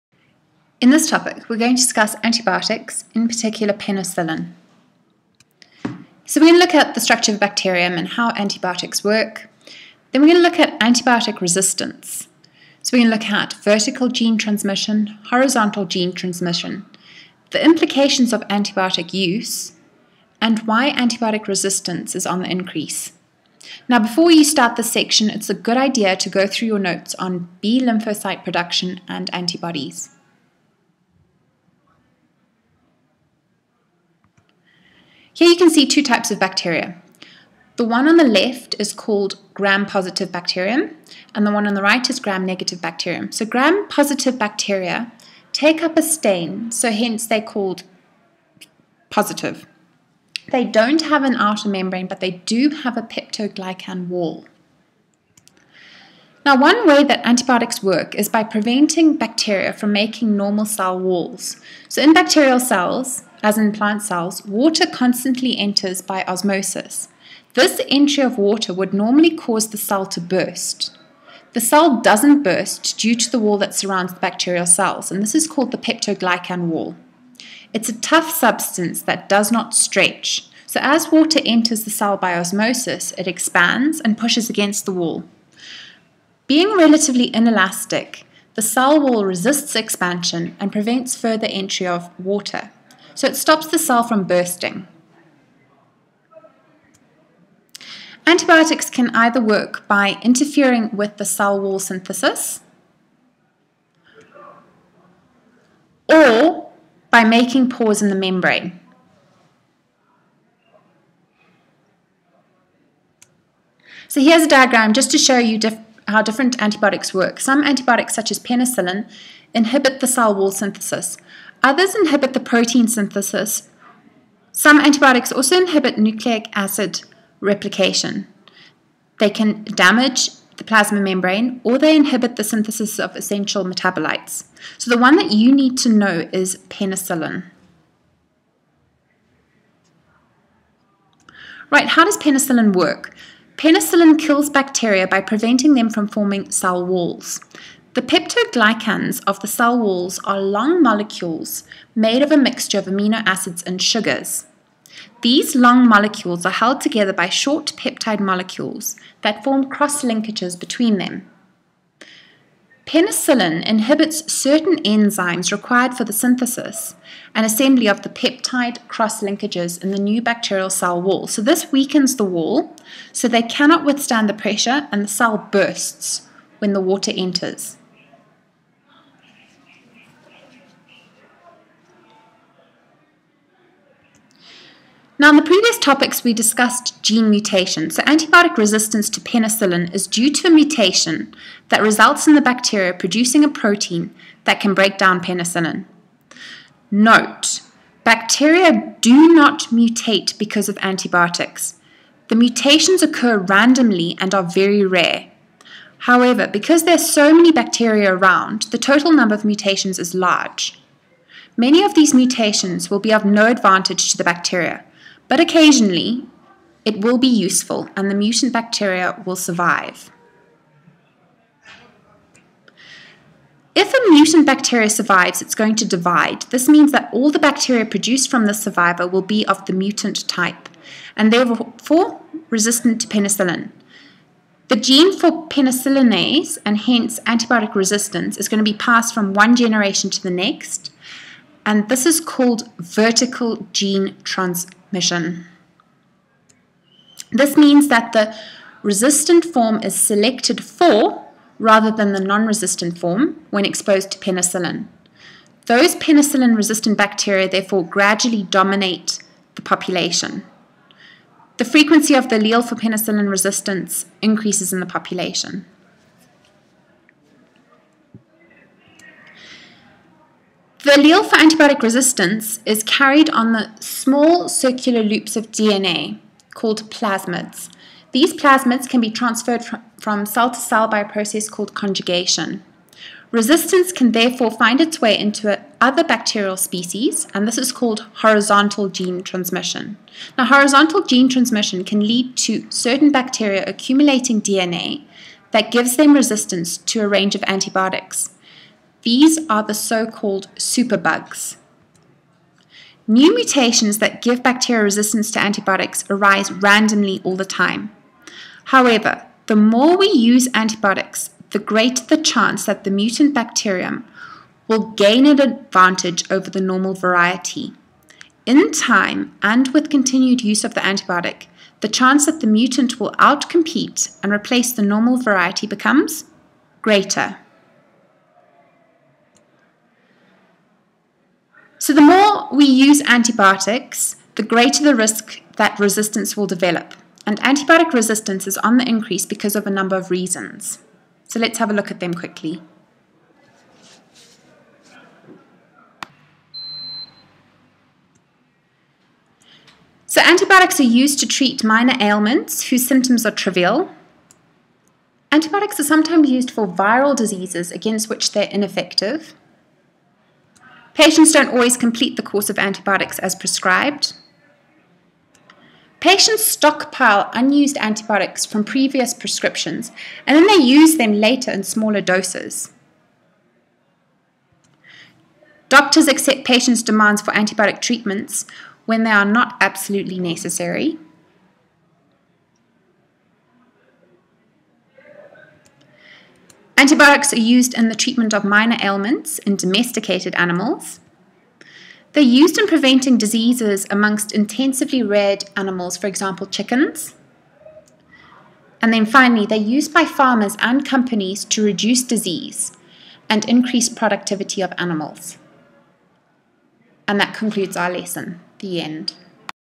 A level. S.4. Antibiotics and penicillin (Ms Cooper)
Structure of a bacterium
How do antibiotics work?
Antibiotic resistance
What is vertical gene transmission?
What is horizontal gene transmission?
What are the implications of antibiotic use?
Why is antibiotic resistance on the increase?